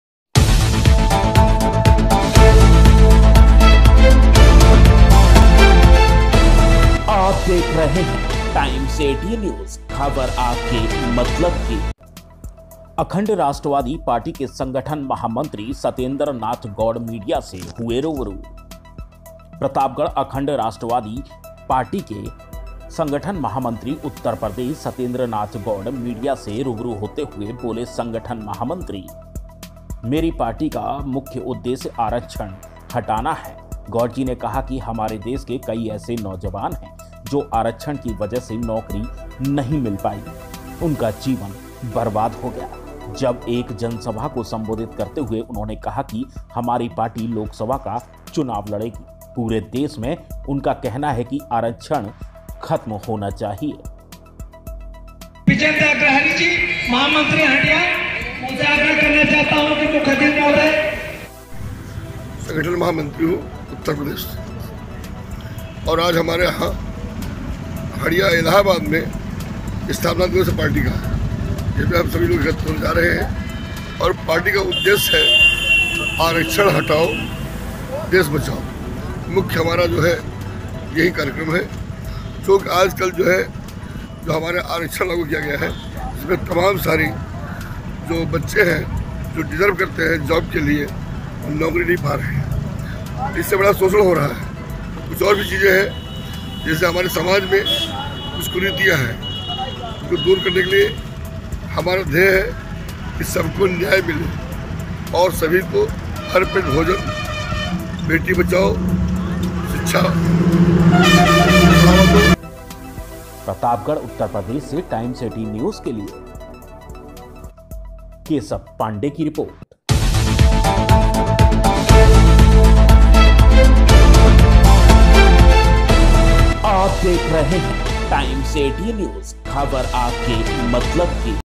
अखंड राष्ट्रवादी पार्टी के संगठन महामंत्री सत्येंद्र नाथ गौड़ मीडिया से हुए रूबरू
रिपोर्ट केशव पांडेय